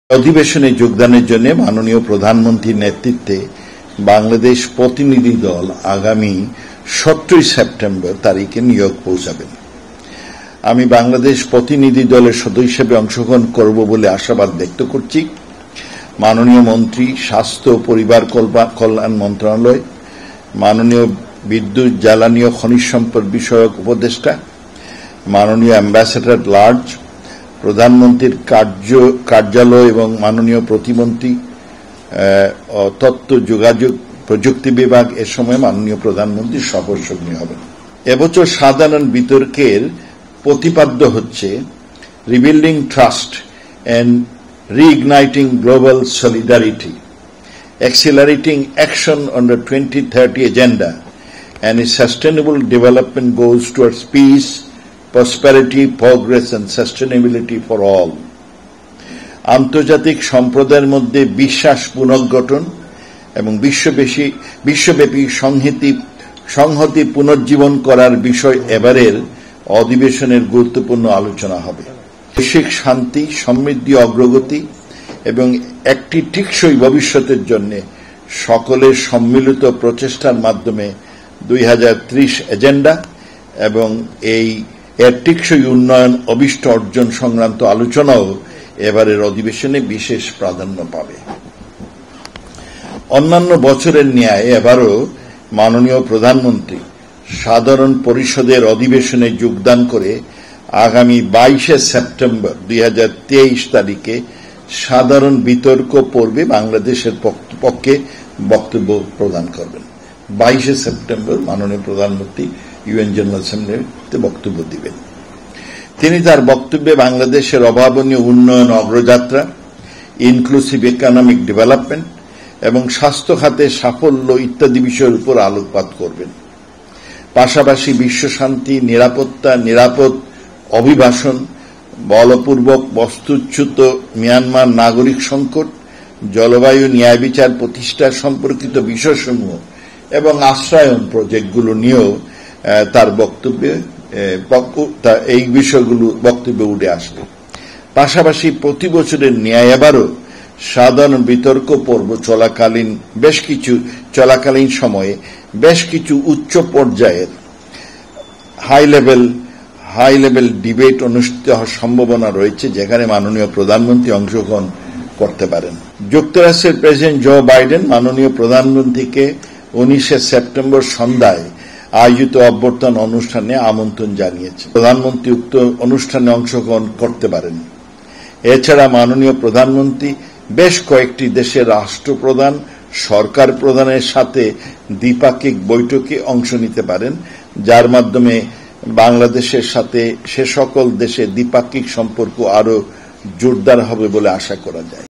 জাতিসংঘ অধিবেশনে যোগ দিতে ১৭ সেপ্টেম্বর দেশ ছাড়বেন প্রধানমন্ত্রী _ PM Sheikh Hasina _ AK Abdul Momen
জাতিসংঘ অধিবেশনে যোগ দিতে ১৭ সেপ্টেম্বর দেশ ছাড়বেন প্রধানমন্ত্রী _ PM Sheikh Hasina _ AK Abdul Momen_Muhit_Bangla_News